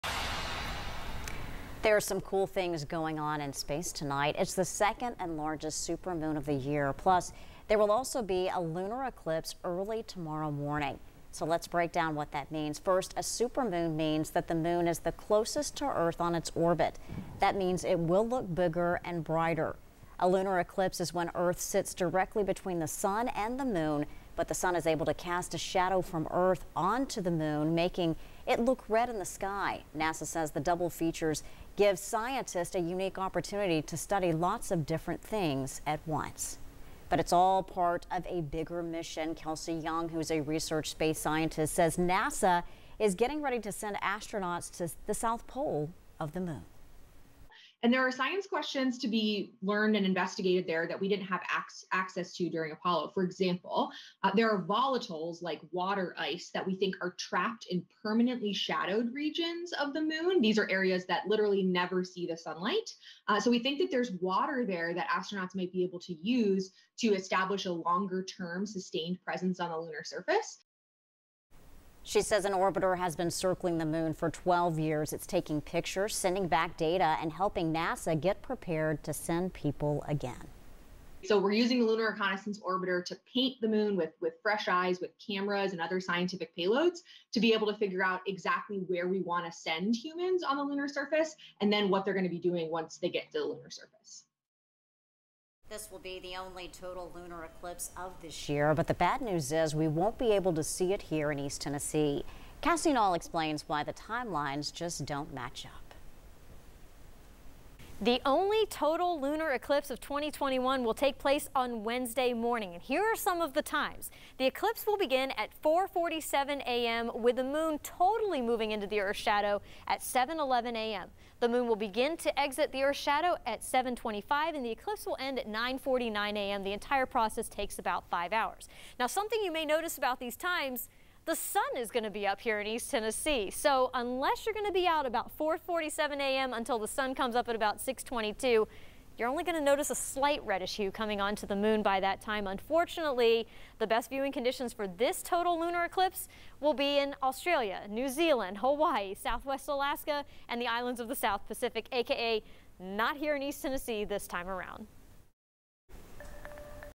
A Supermoon and Lunar Eclipse are happening tonight!
It's the second and largest supermoon of the year.
plus-- there will also be a lunar eclipse early tomorrow morning.